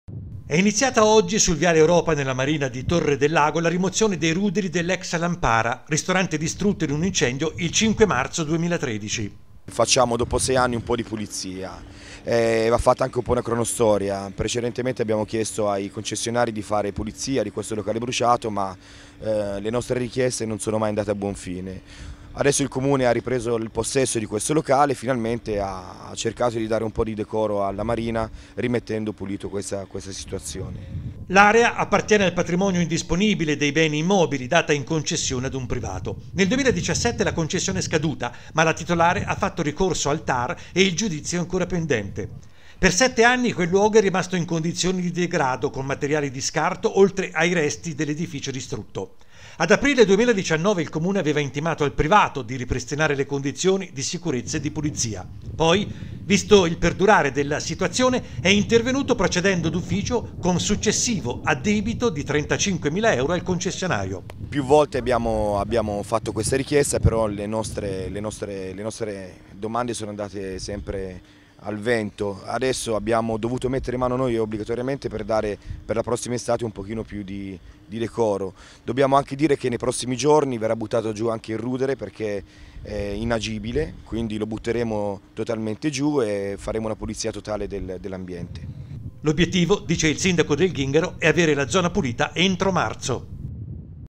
Via le macerie del ristorante andato a fuoco sette anni fa
TORRE DEL LAGO - Lavori in corso sulla Marina di Torre del Lago: è partita questa mattina la rimozione dei ruderi dell’ex Lampara, ristorante distrutto in un incendio il 5 marzo 2013.